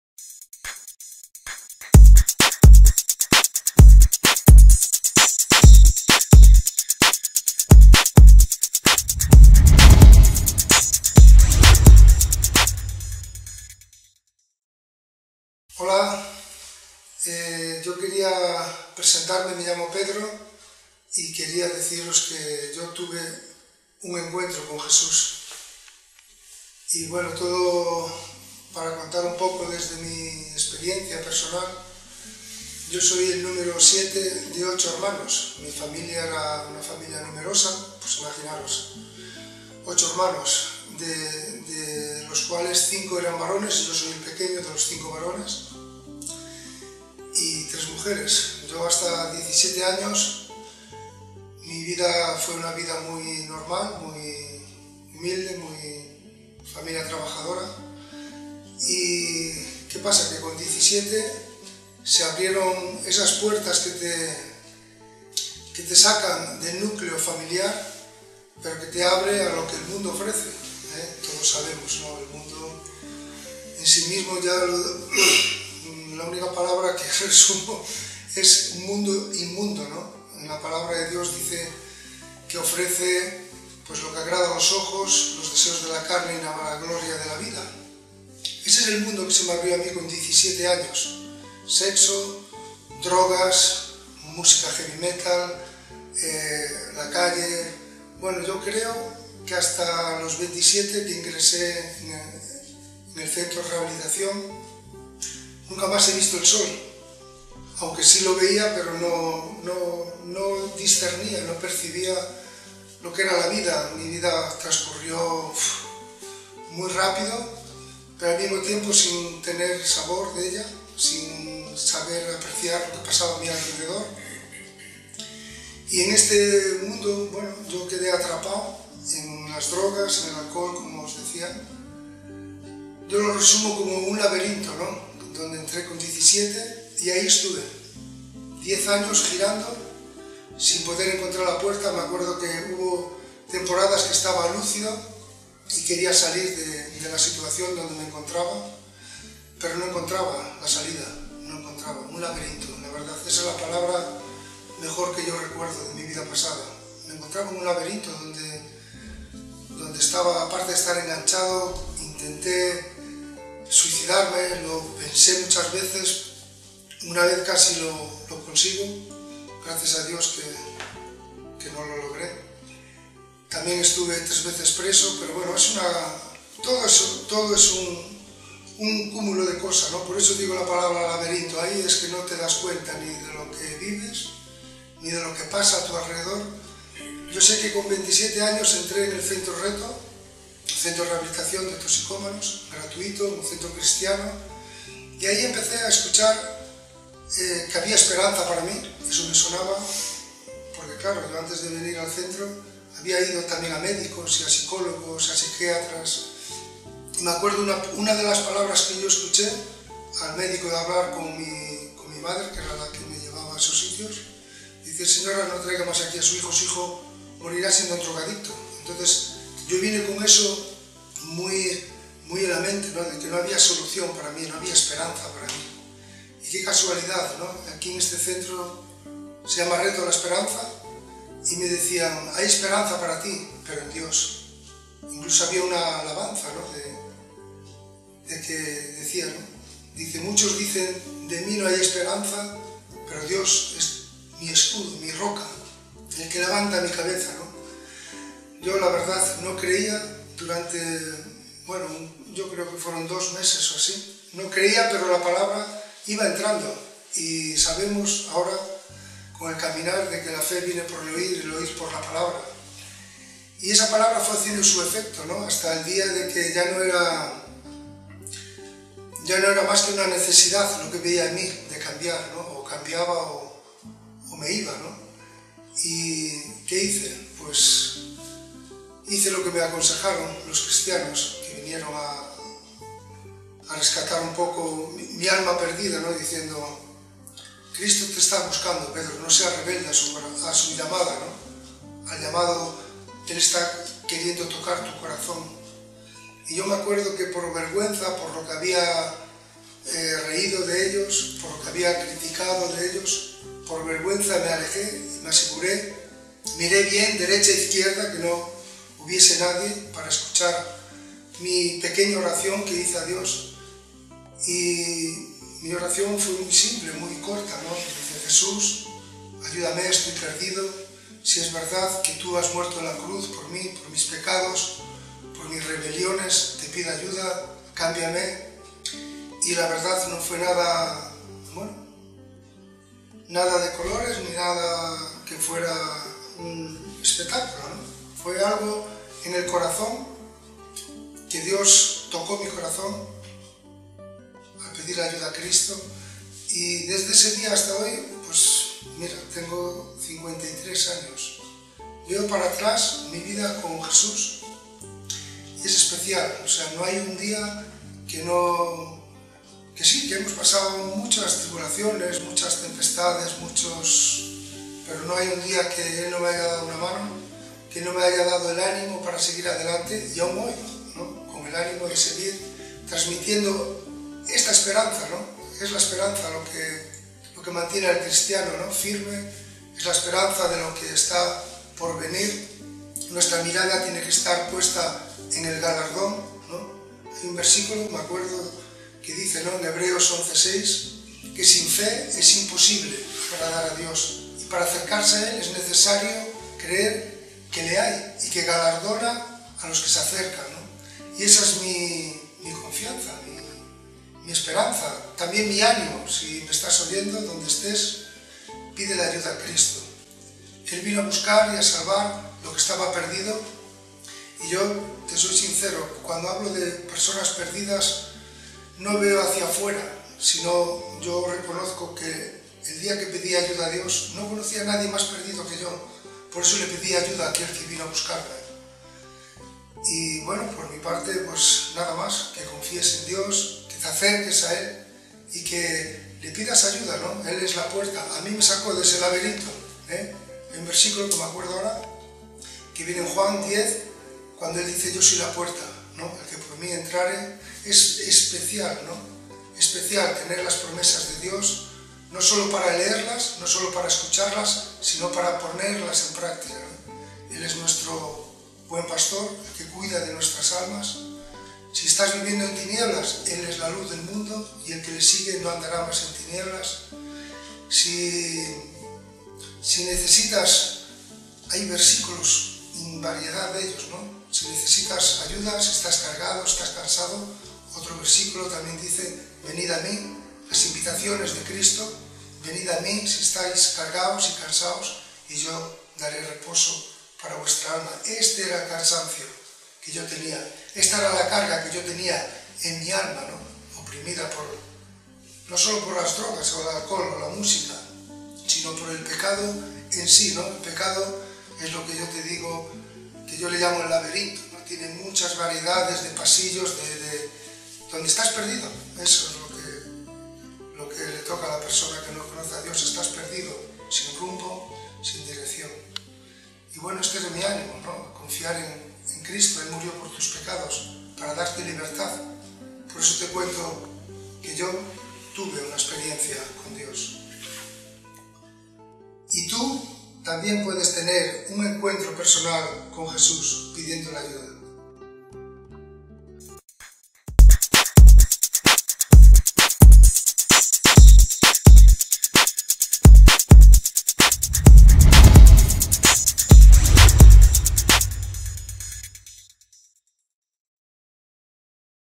28-YOUTUBE#UN ENCUENTRO CON JESÚS (PEDRO ROMERO)
YouTube# Un Encuentro con Jesús
Desde nuestro canal cristiano “Luz a las Naciones”
Os presentamos estos vídeos de testimonios
de hombres y mujeres que hemos tenido un encuentro con Jesús
que ha cambiado nuestras vidas para siempre.